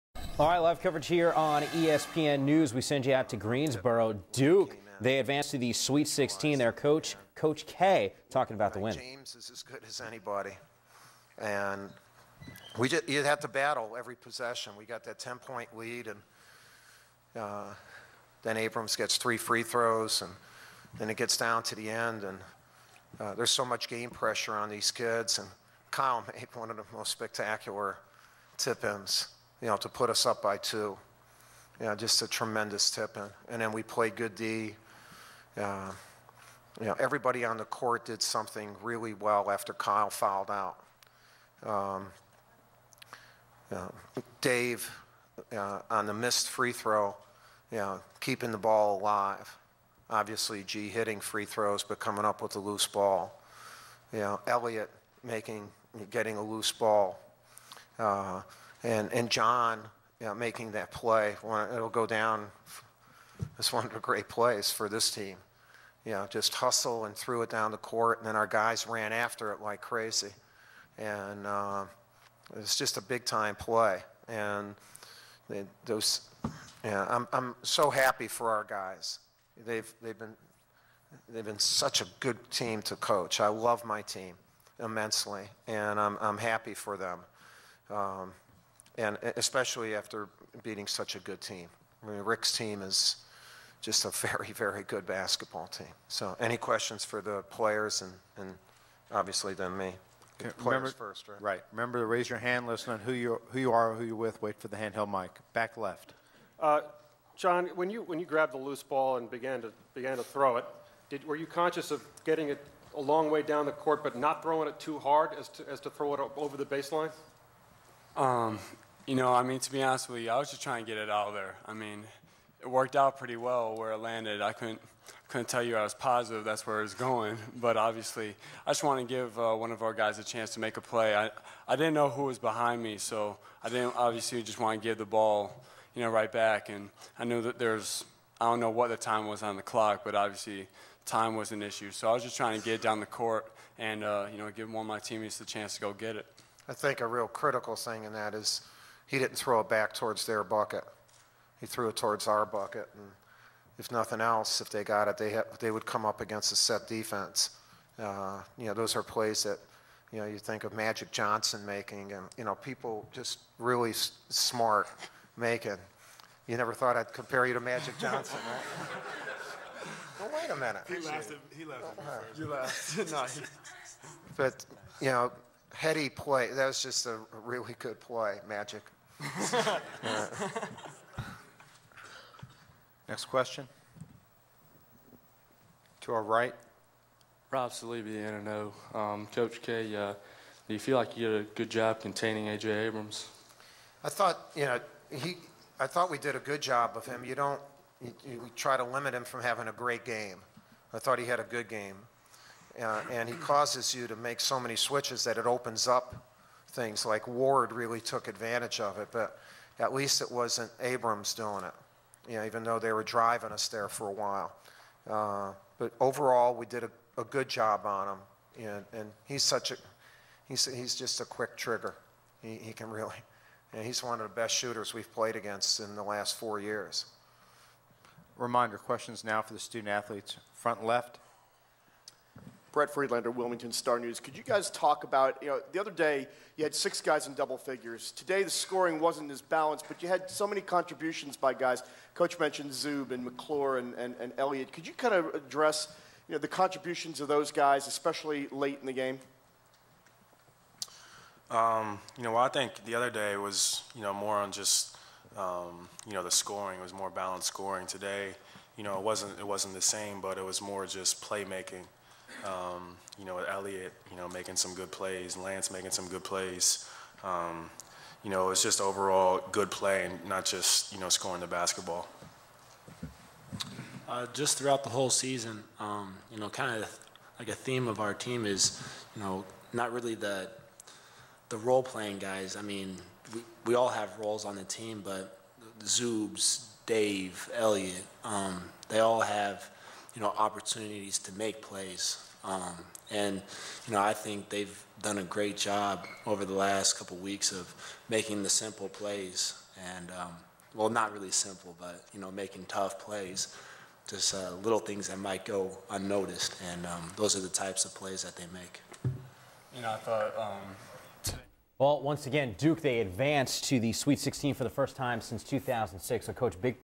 Jon Scheyer Press Conference 3/21/09
Jon, Gerald Henderson, Kyle Singler and Coach K field questions from the media following Duke's second-round victory over Texas on March 21, 2009.